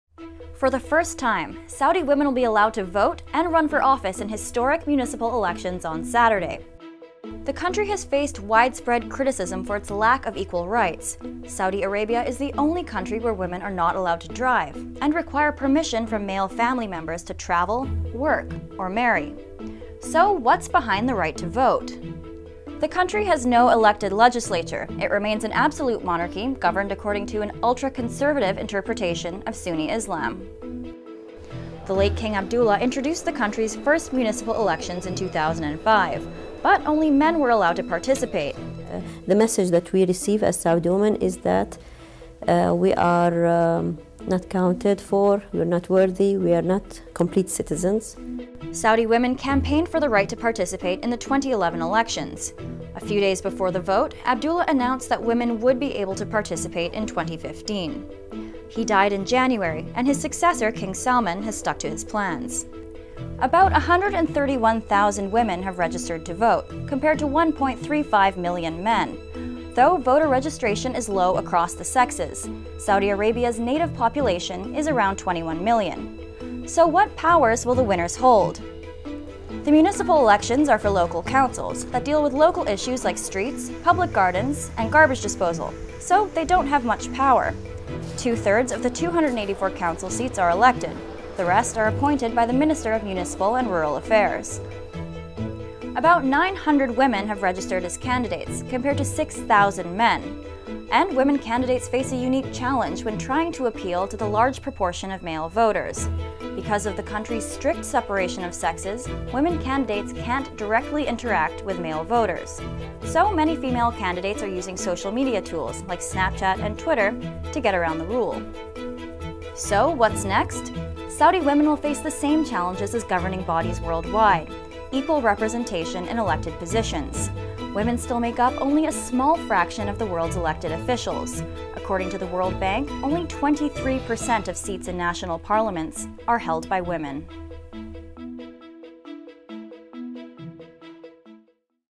Saudi Women Get the Vote: An Explainer
Women in Saudi Arabia will be allowed to vote and run for office in historic municipal elections on Saturday. WSJ's Niki Blasina explains what's behind the right to vote, and the unique challenges female candidates face. Photo: AP.